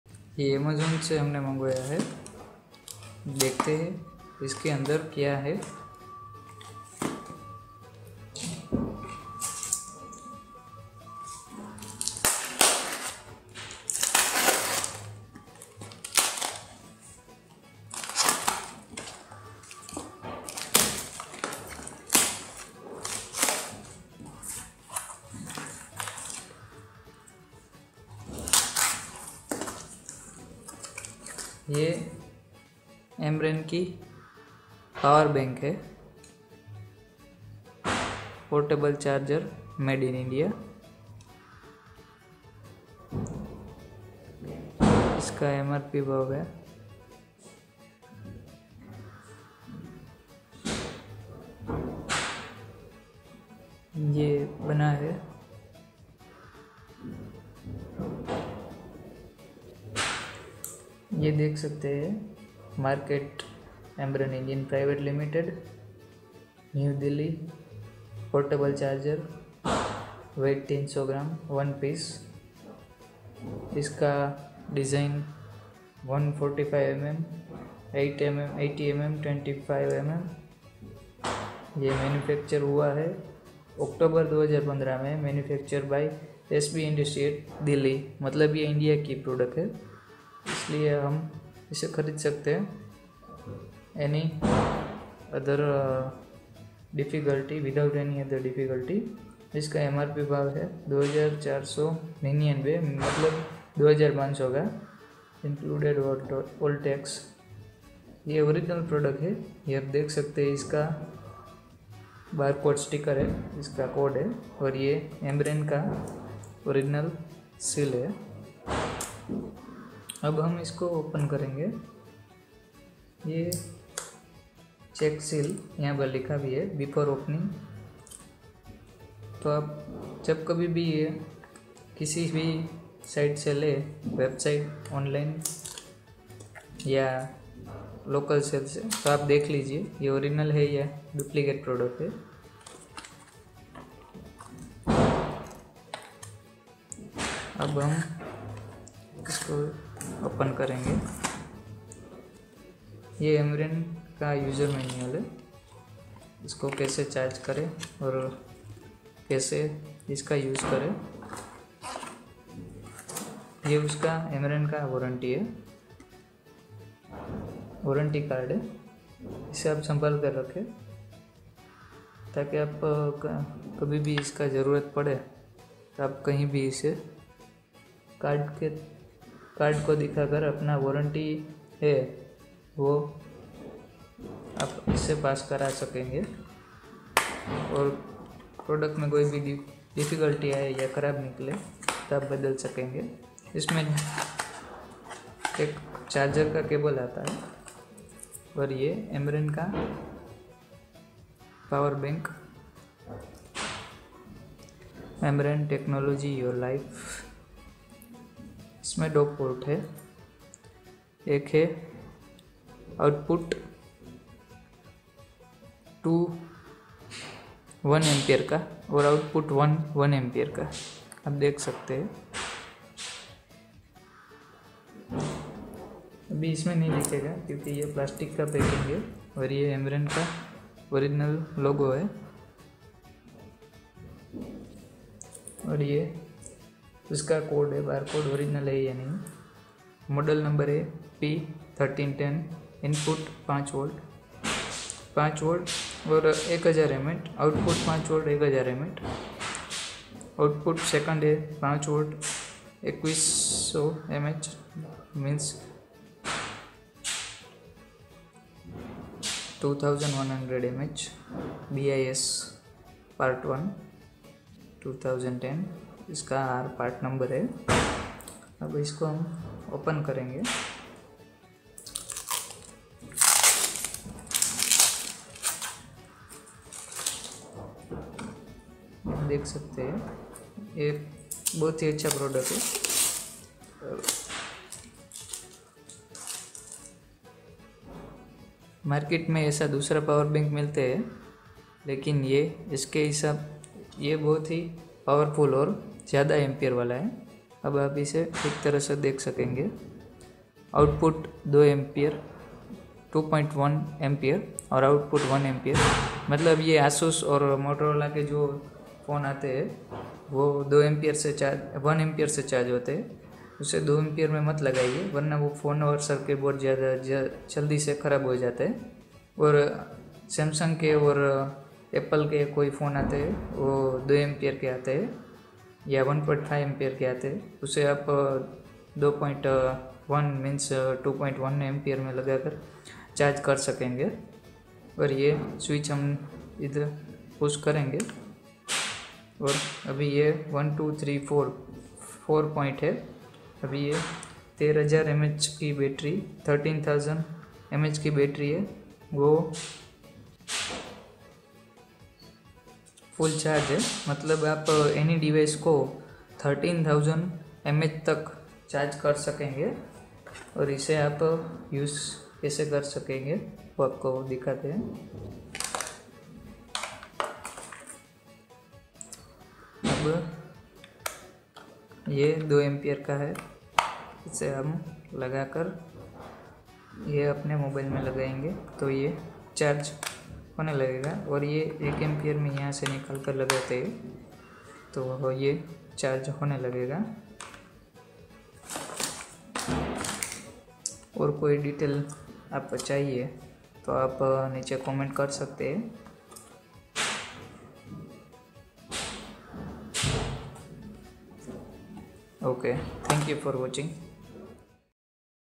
Ambrane Power Bank P 1310 13000mAh unboxing
only in Rs.1000 with one year Ambrane India warranty
I have personally used from 2 years.